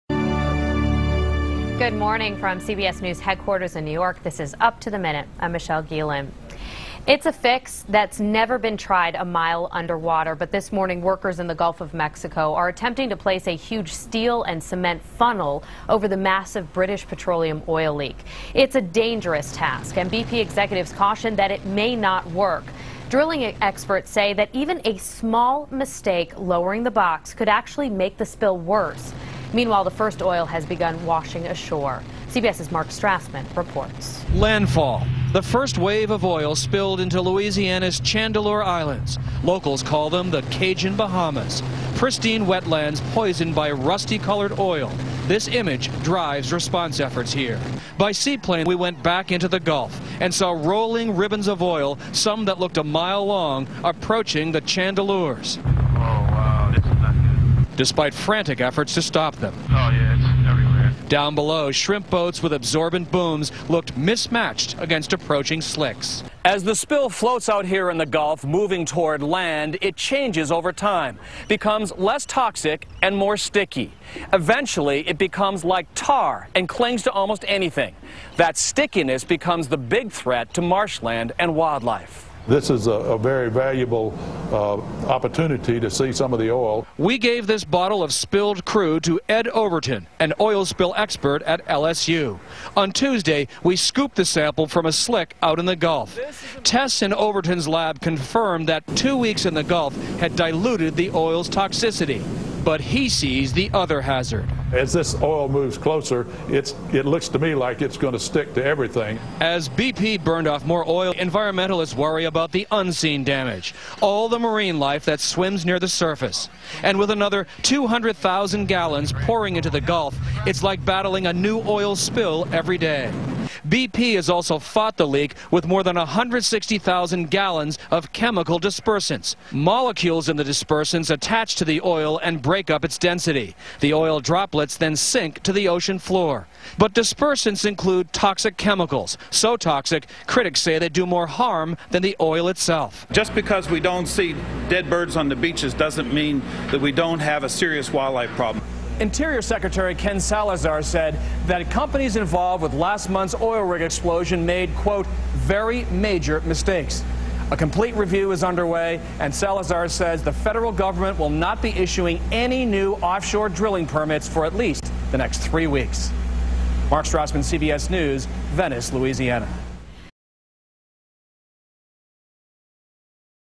Oil Spill Disaster 2010 (part44) - Plan To Cap Leaking Oil Could Make Things Worse!
Footage about the greatest oil disaster of all times (2010 Gulf)
Watch at ur Planet like it would be ur Child!
dont close ur eyes! do ur part!
be nature citizan!